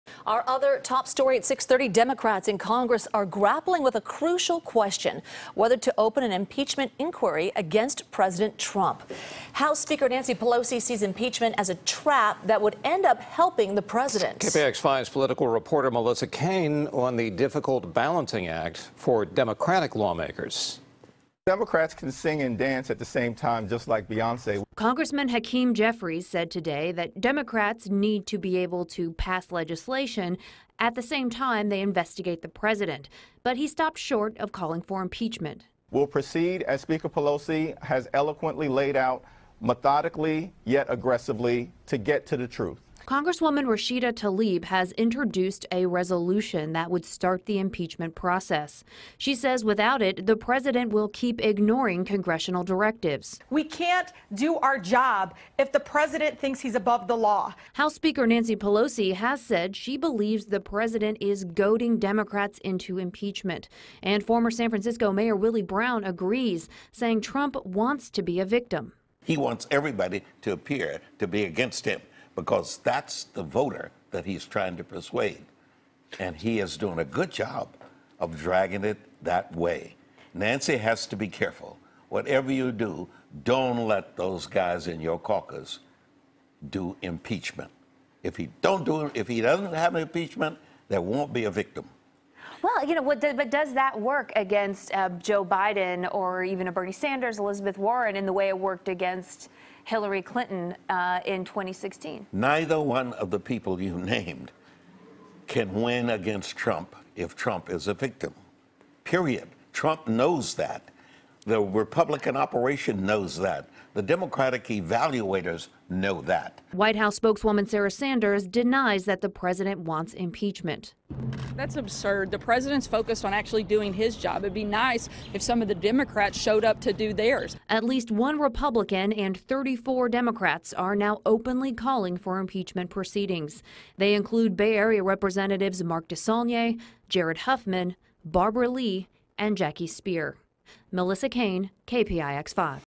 Congressional Democrats Grapple With Question Of Impeachment
Democrats in Congress are grappling with a crucial question: whether or not to open an impeachment inquiry against President Trump. Melissa Caen reports. (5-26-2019)